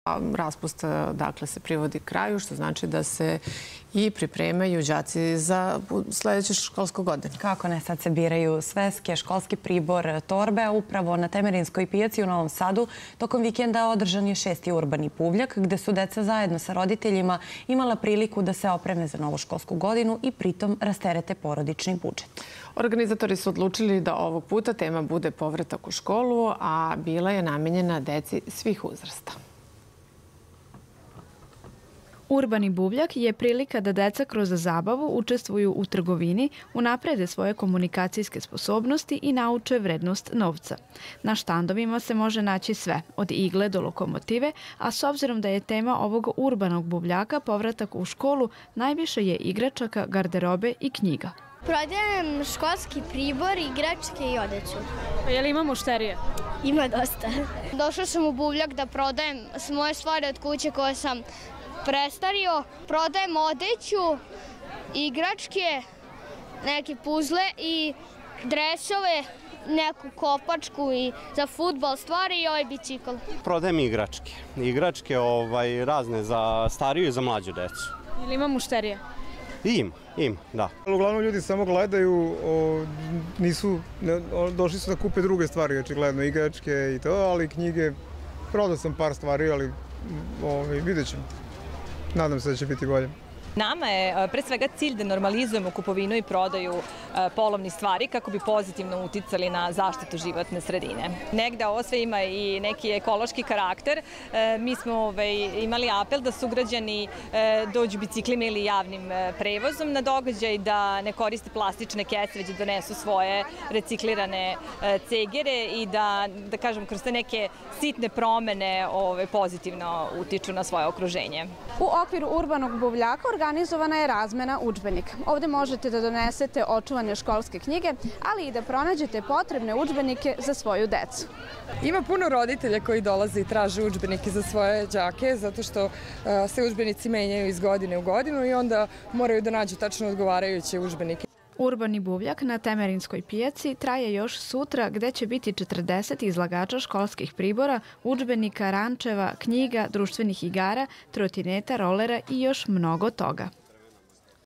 Da li je sve spremno za početak nove školske godine
Školarci se polako pripremaju za novu školsku godinu, ali poslednje pripreme za rad realizuju se i u okviru školskih objekata.  Da li je sve u potpunosti spremno za 1. Septembar?  Da li je sve spremno za početak nove školske godine?